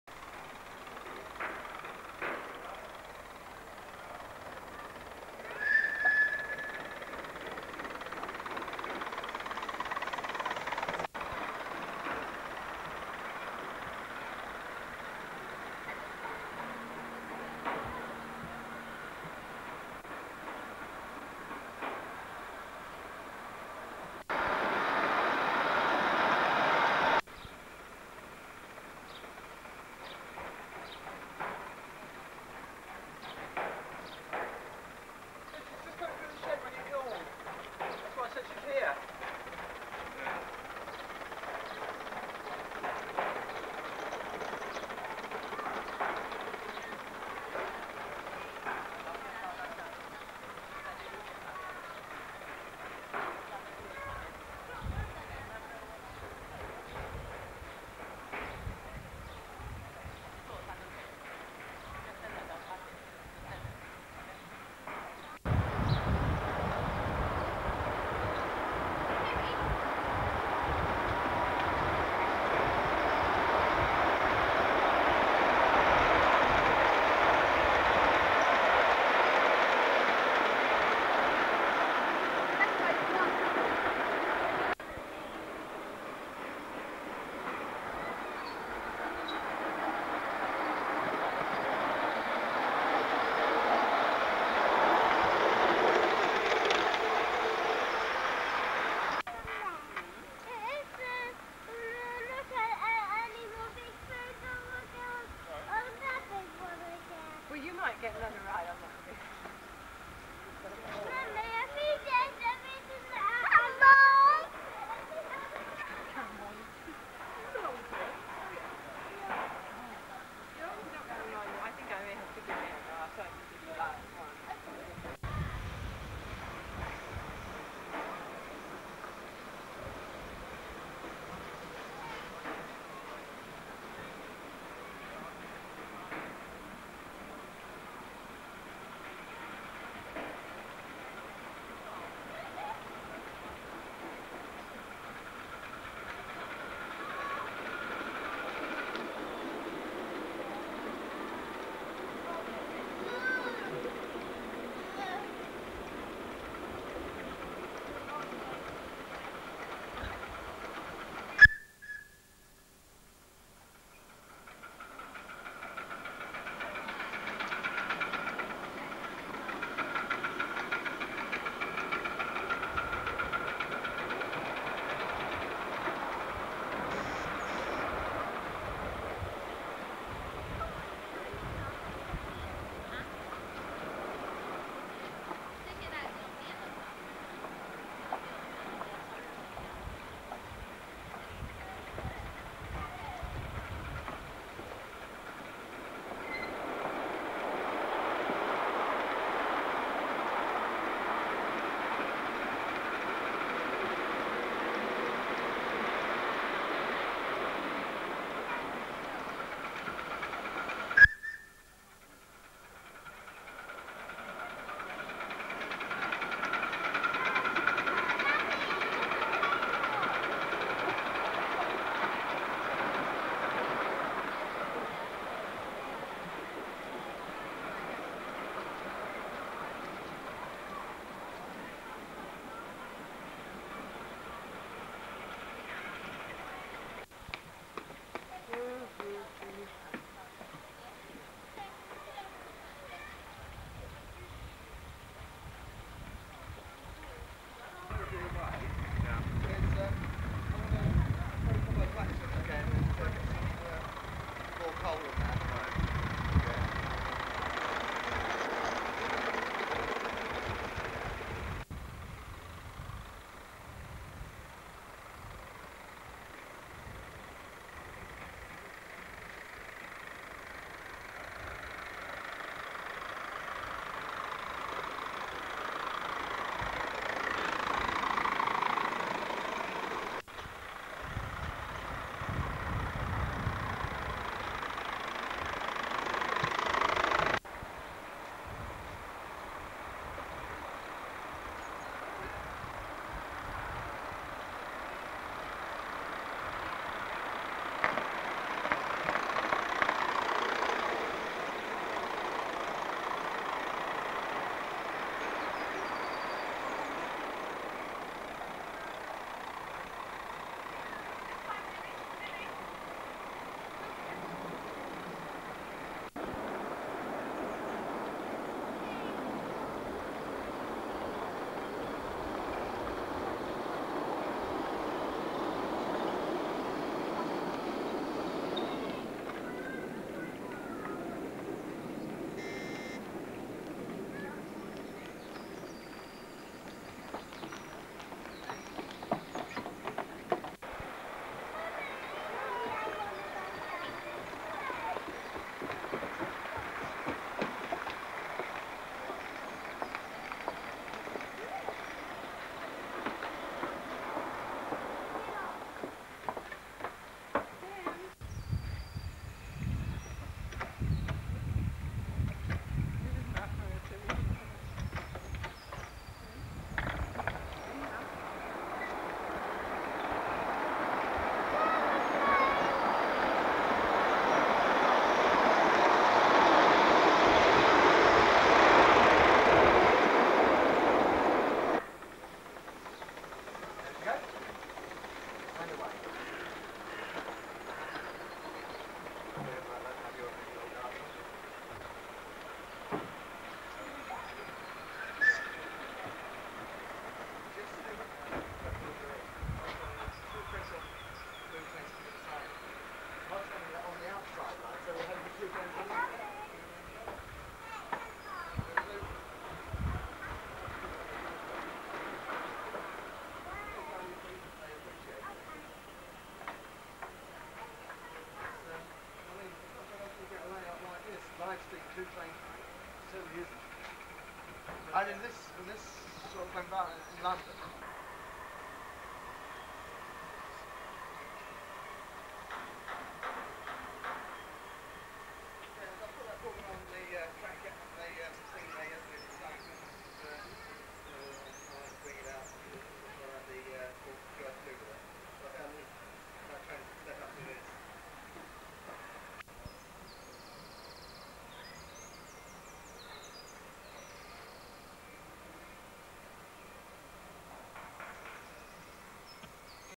London Toy & Model Museum 1985
This video was taken during my visit to the London Toy Museum in 1985.  Sadly, the London Toy Museum was closed in the late 1990's following the death of the owner, Kazuaki Fujita, a toy collector who died in 1995. It was during my visit to the London Toy Museum that I realised it was possible to run seven and a quarter inch ride on train around a reasonably tight radius, hence my current backyard set up which has a four metre radius.